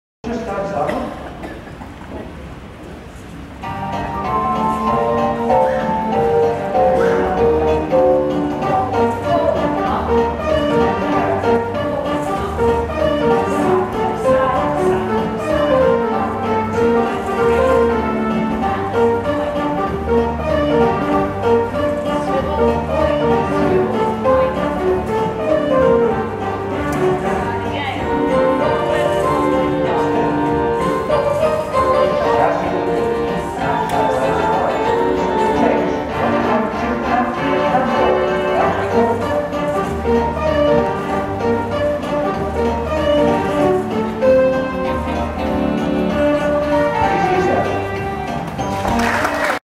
Breken Quickstep demonstrated by Andrea and John at Rutherglen 2016
New Sequence Dance Breken Quickstep demonstrated by Andrea and John at Rutherglen 2016